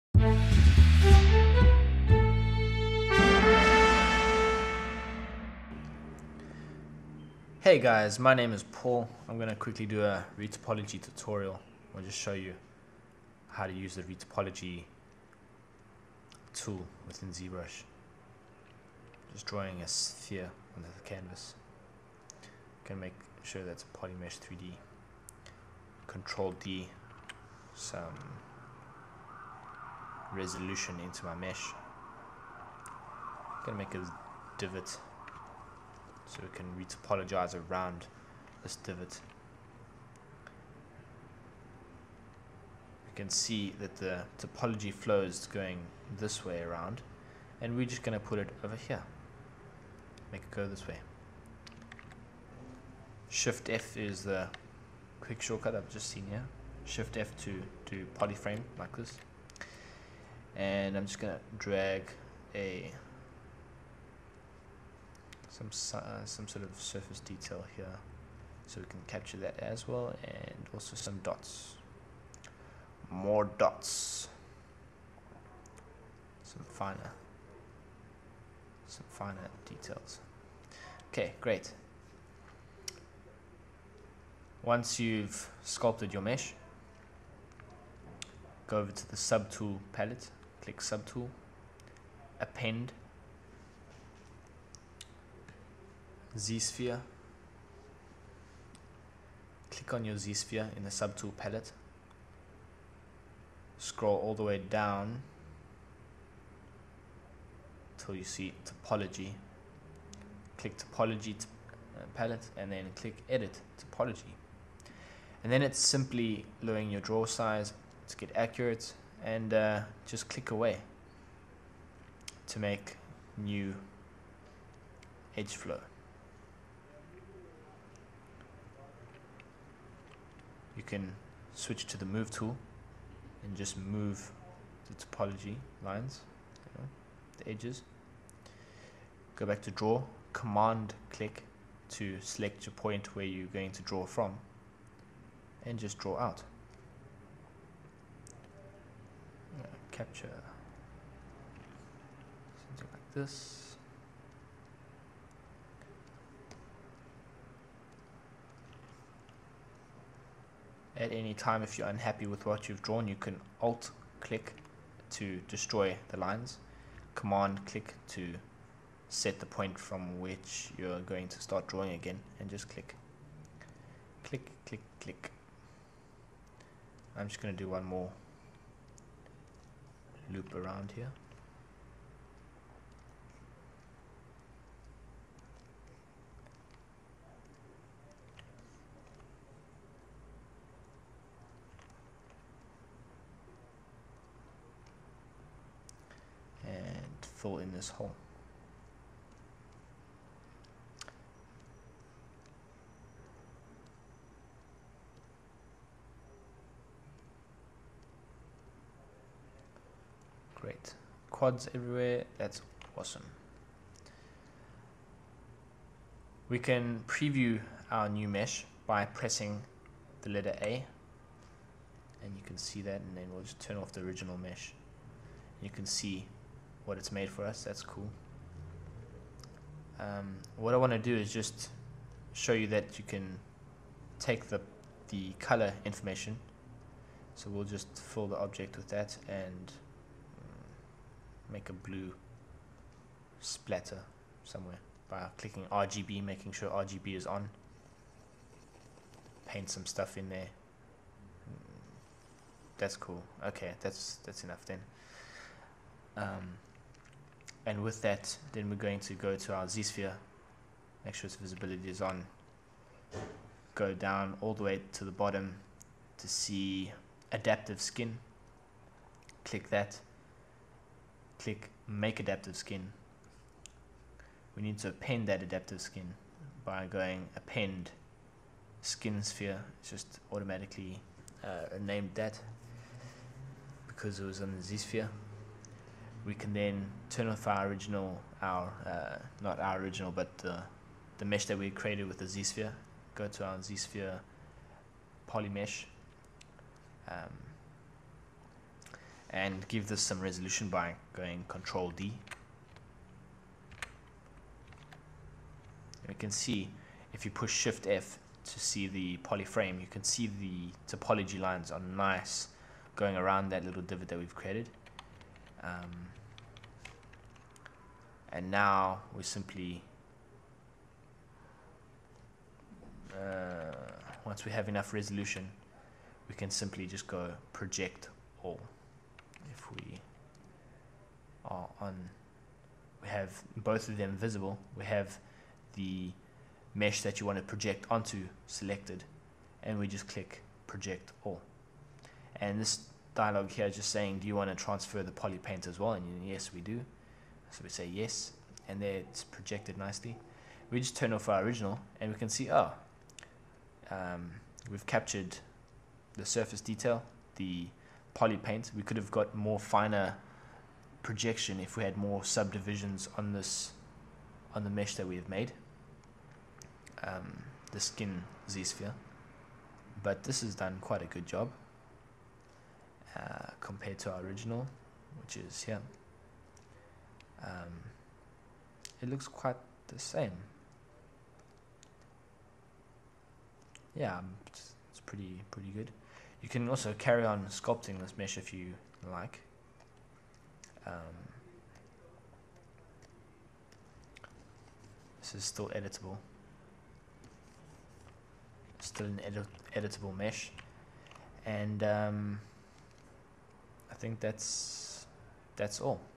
Zbrush retopology tutorial
A quick tutorial on how to retopologize using zspheres in zbrush.